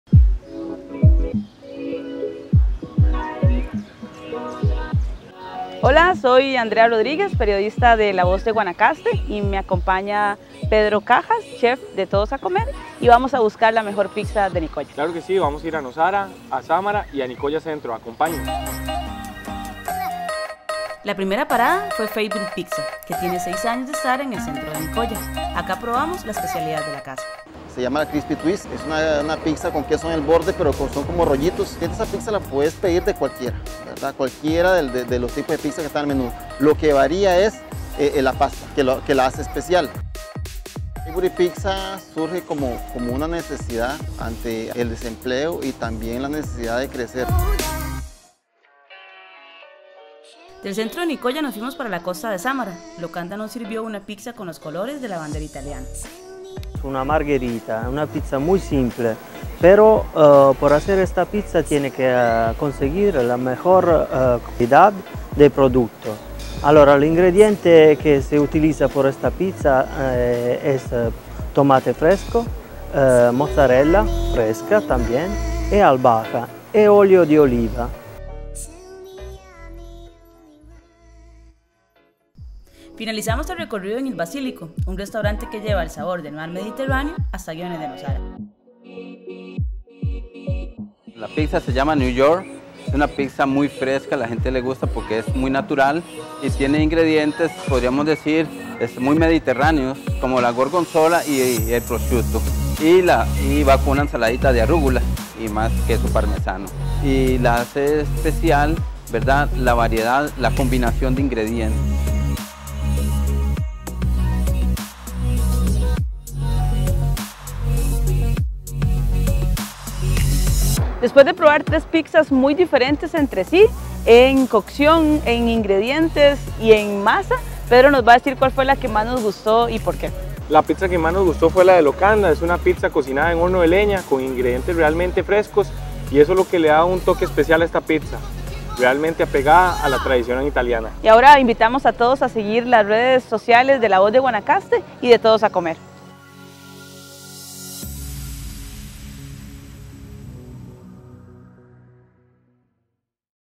¿Dónde está la mejor pizza del cantón de Nicoya?
La Voz de Guanacaste y Todos a Comer visitamos tres pizzerías en el cantón y un chef eligió la mejor de ellas